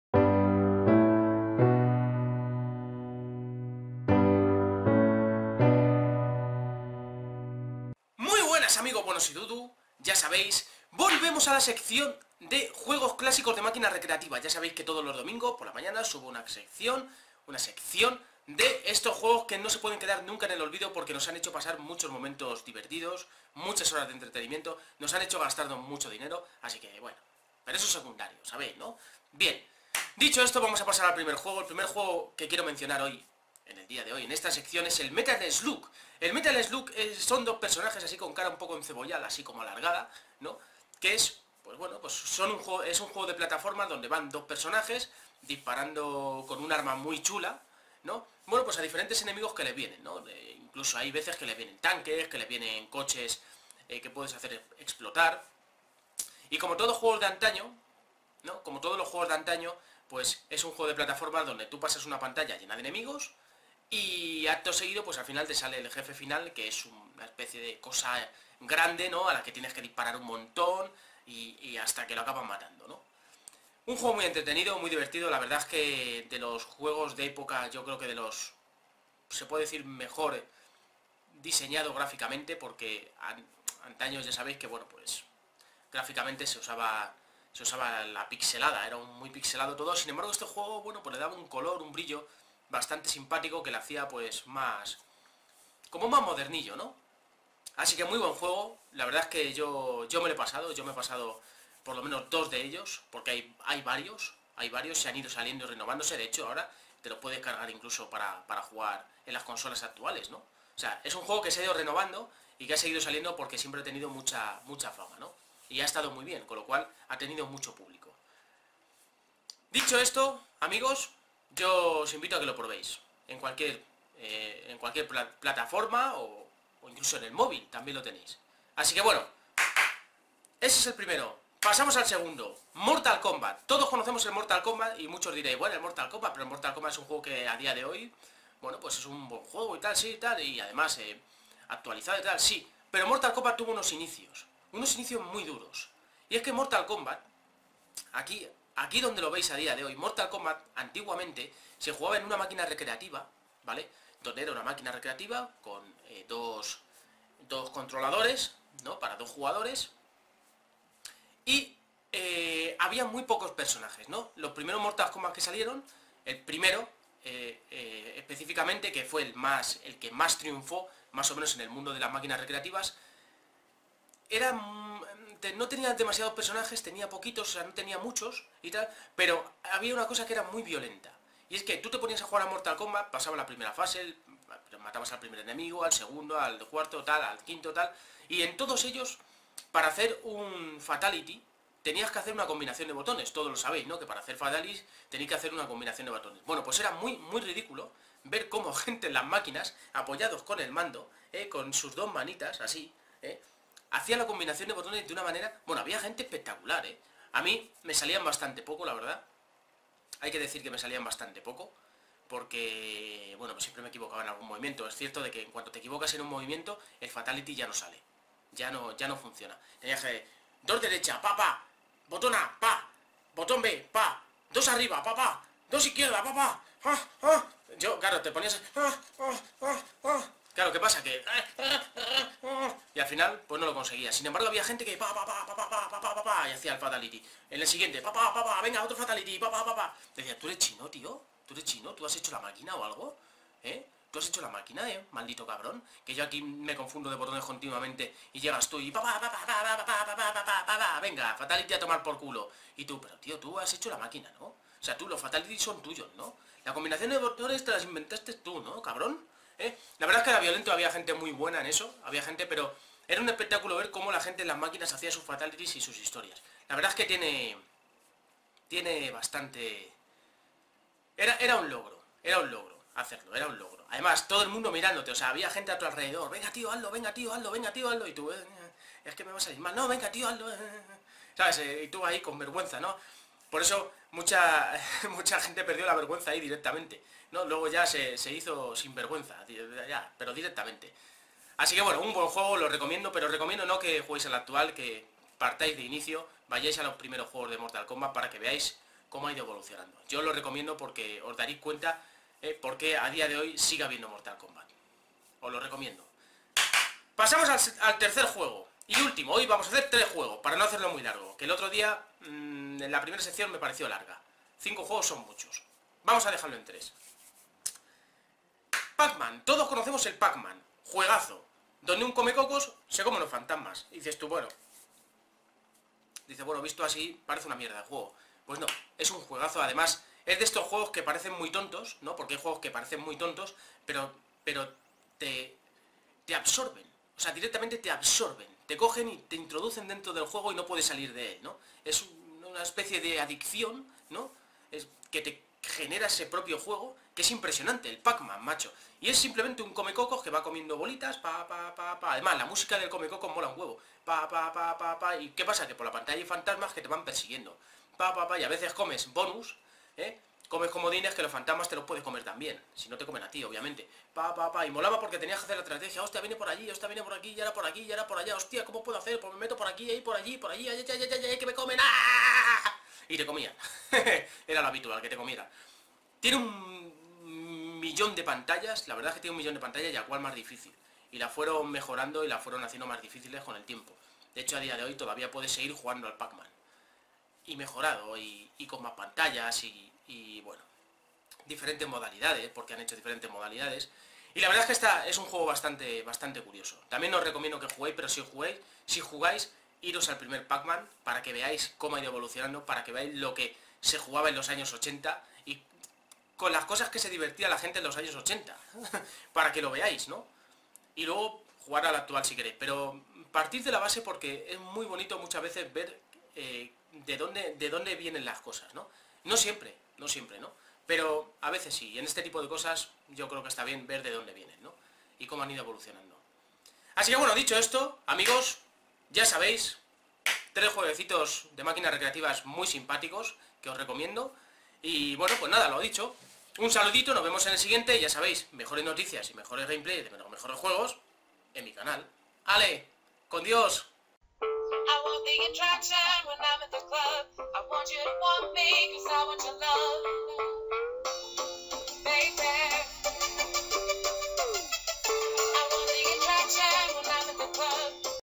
Juegos clasicos de máquinas recreativas 2ª
En esta ocasión veremos otros 3 juegos clásicos de máquinas recreativas para rememorarlos y que nunca pasen al olvido.